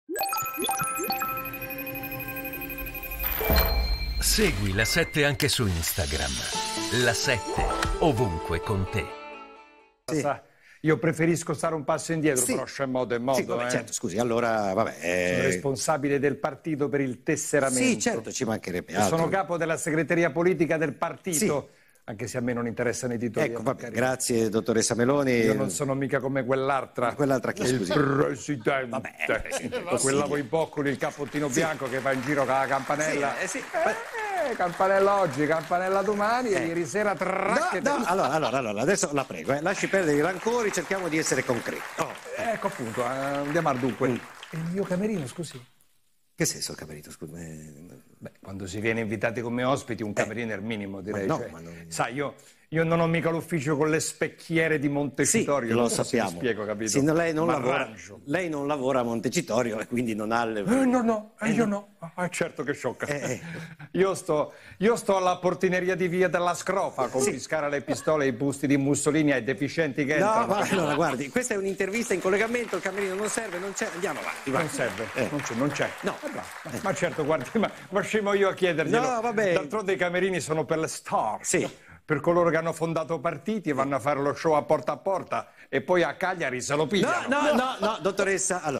Luca e Paolo, Arianna Meloni la sorella che requisisce i busti del duce
Le parodie di Luca e Paolo